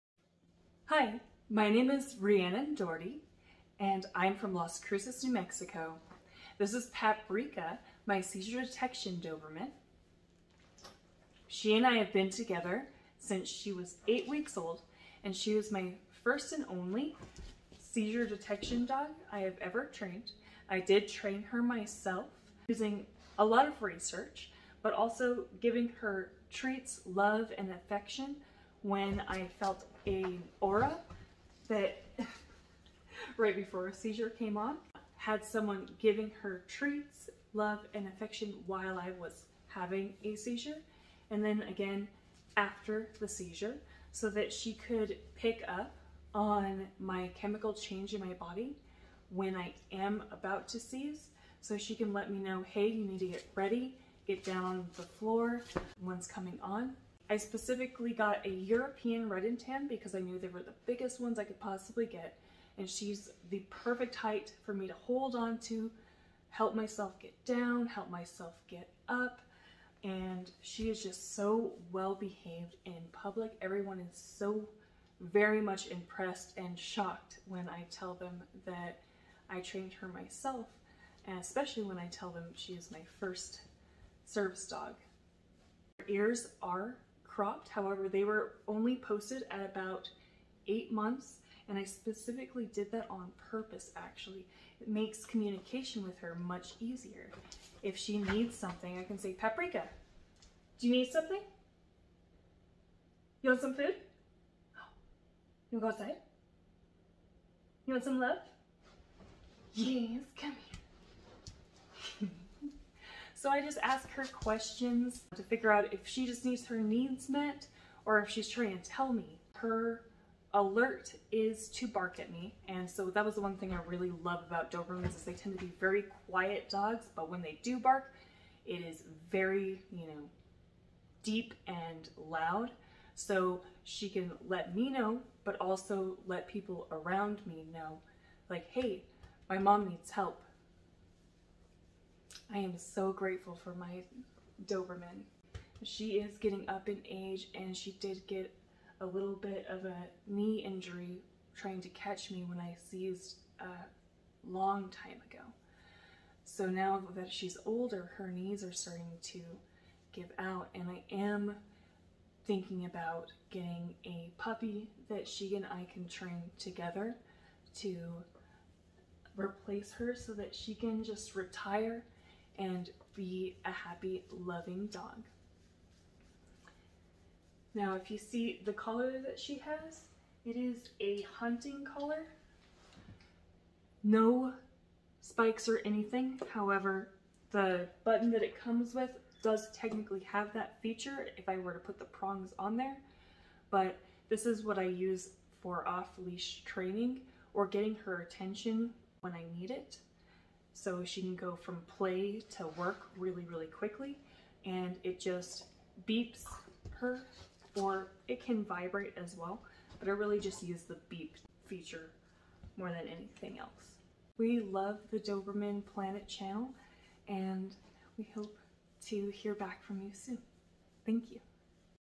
Doberman Planet interest card submission
My European Red Doberman, Paprika, is my Seizure Detection Service Dog. I have had her since she was 8 weeks and trained her myself. She is my first service dog. I chose the Doberman breed for many reasons, but most importantly for their tendency to be so owner focused and easy to train off leash.

I sustained a traumatic brain injury when I was 4 years old, causing me to lose my entire left parietal lobe and surrounded area. I had to relearn how to walk, talk, and get my nervous system back to as normal as possible. This TBI resulted in a severe seizure disorder I have been battling with ever since. I got Paprika in 2017 as I prepared myself to live by myself for the first time. Paprika's job helps me recognize the frequency of my seizures and prepares me in hopes I can decrease their severity and prevent injuries.

Paprika gets so many compliments on how well trained she is. When people ask me where I got her trained and I tell them I trained her myself and that she is my first service dog, they are amazed. I did a lot of research before getting Paprika, but the most unique part of our relationship is how we communicate with one another. Her ears are cropped, but I purposely stopped posting them at 8 months so she is easier for me to read. When she is trying to get my attention, I ask her questions, and her ears direction changes when I ask the right question. This way I know which of her basic needs need to be met or if she's trying to warn me that a seizure is coming on. Her alert signal is to bark at me, but she will try to get me to sit down and get ready before she barks if she can. This way, I know it is coming, and I am prepared.

My TikTok channel is @FaeForDays, primarily containing outdoor adventures with Paprika.
I have a tripod, and I specifically choose my phones based on camera quality.
I'm not sure if my interest card went through as there were submission loading issues so I thought I'd post this to my channels for good measure 💪